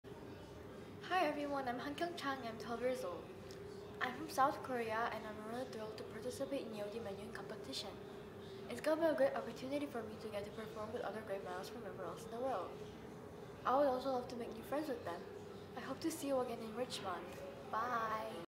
Hankyoung Chang, 12, South Korea
Hankyoung Chang began playing the violin at four years old. At eight years old, she was admitted to the Pre-College division of the Julliard School, where she studies with Professor Hyo Kang. Her awards include the 2018 Arkady Fomin Scholarship, and the 2019 Sejong Soloists Young Artist Merit Scholarship. In 2018, she became the youngest recipient of the Stradivari Society of Chicago. She was a student in Hic et Nunc 2019! Music Festival and participated in masterclasses with Vadim Repin and Clara-Jumi Kang. Hankyoung has also participated in masterclasses with Vadim Gluzman and Kyoko Takezawa. Besides playing the violin, Hankyoung loves reading and drawing.